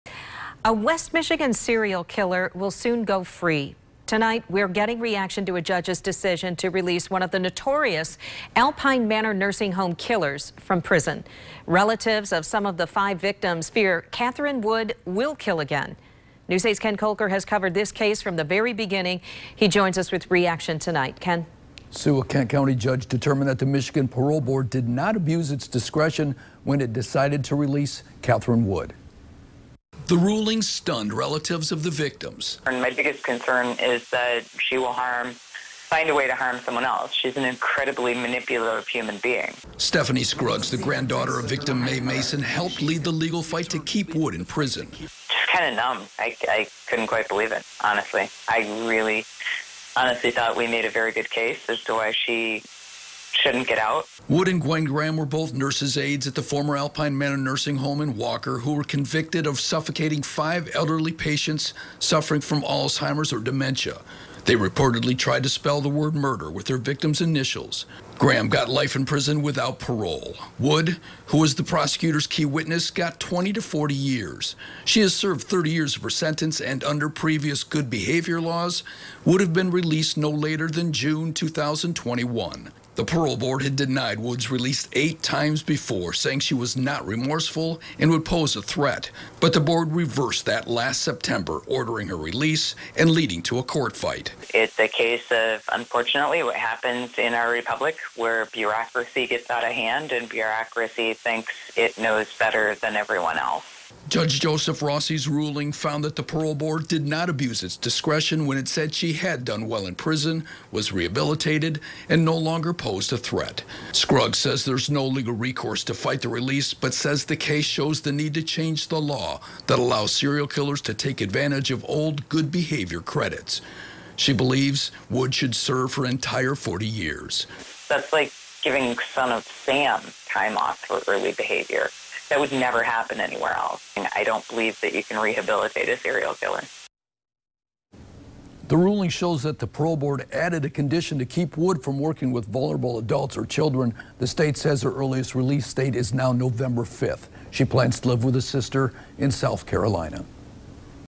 Nursing home killer granted parole
A Kent County judge has ruled that Alpine Manor nursing home killer Catherine Wood should be released on parole. (Oct. 16, 2019)